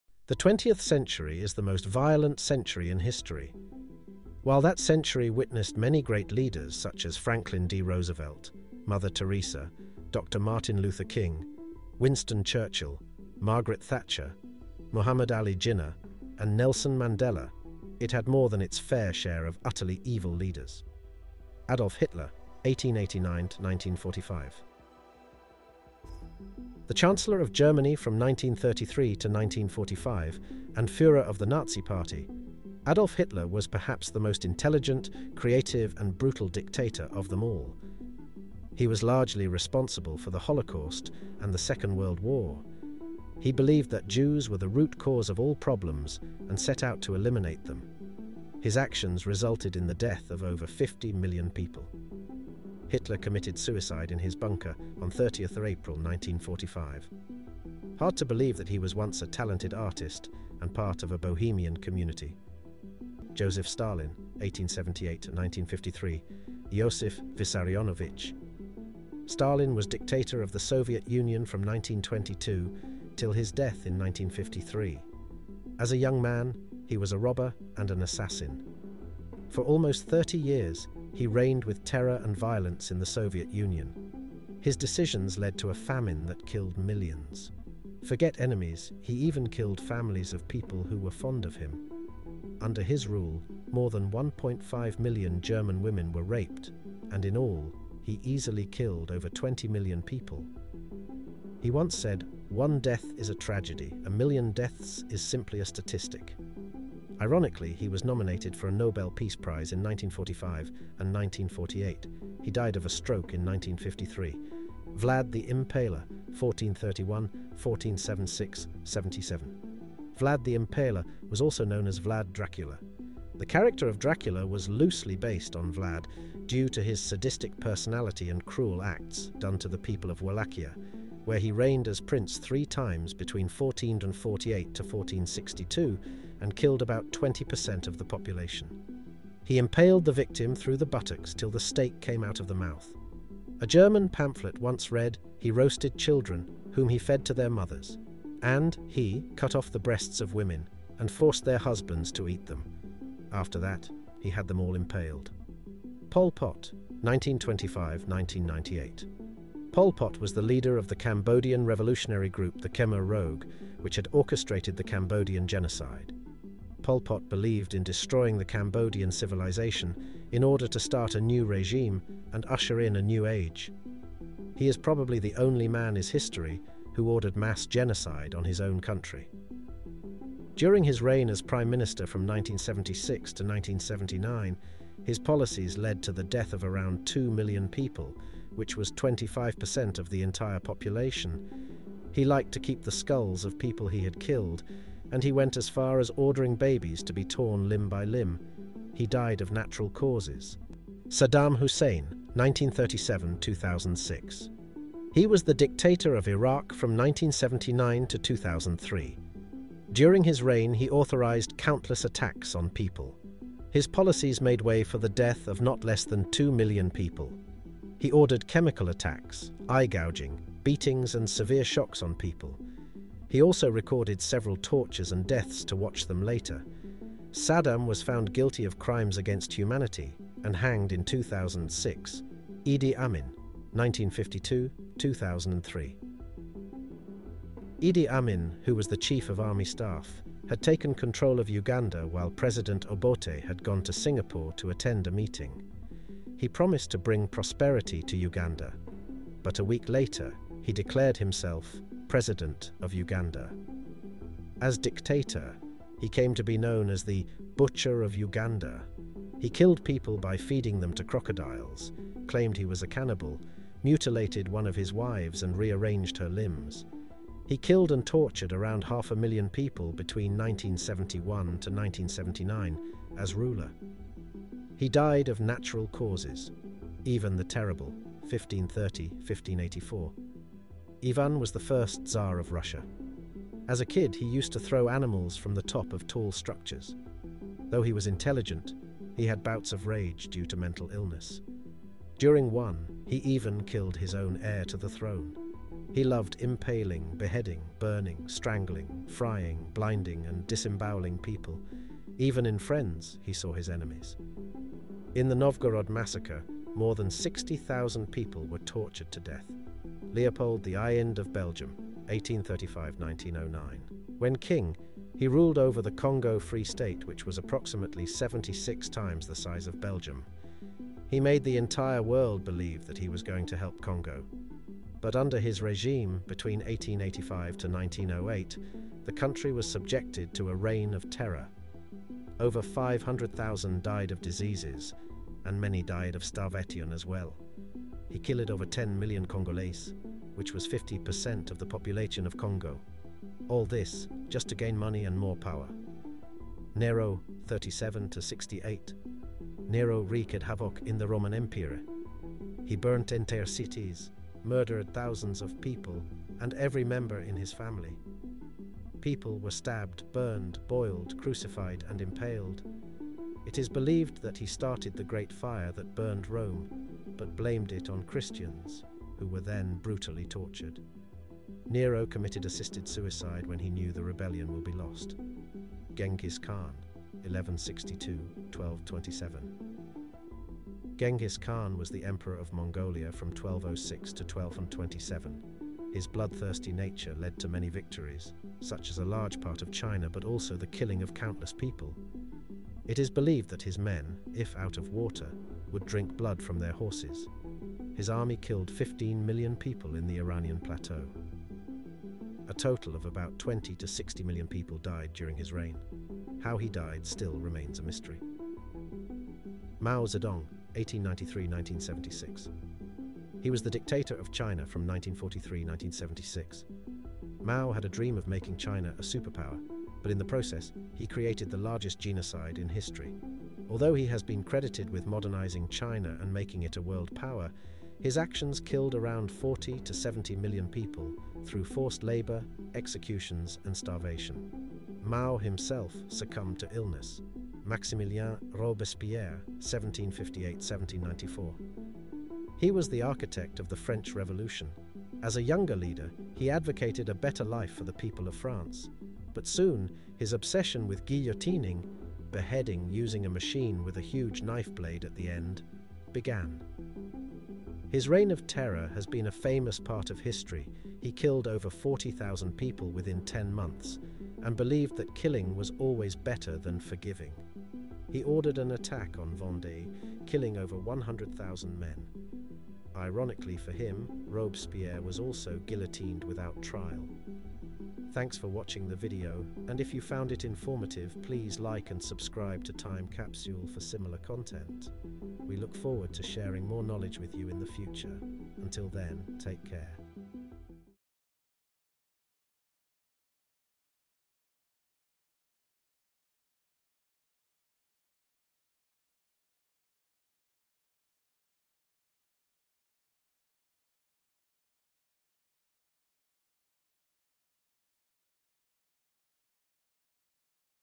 Most RUTHLESS Dictators and What they Did
The 20th century is the most violent century in history. While that century witnessed many great leaders, such as Franklin D. Roosevelt, Mother Teresa, Dr. Martin Luther King, Winston Churchill, Margaret Thatcher, Muhammad Ali Jinnah, and Nelson Mandela, it had more than its “fair” share of utterly evil leaders.